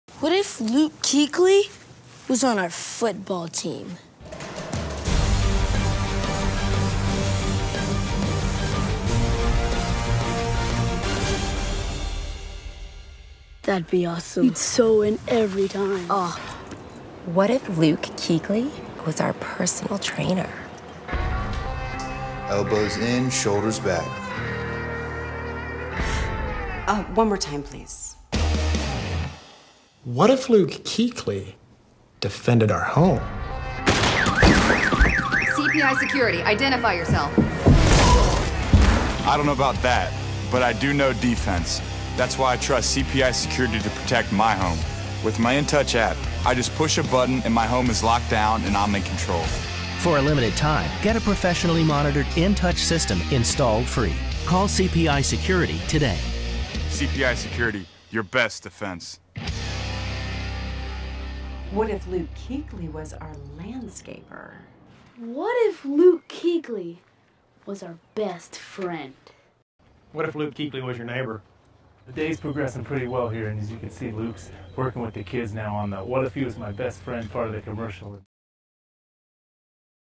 What if Luke Kuechly was on your team / personal trainer - CPI Security
What if Luke was on your peewee football team? Or your personal trainer? Or defended your home?

Luke knows defense, that's why he chooses CPI Security to protect his home.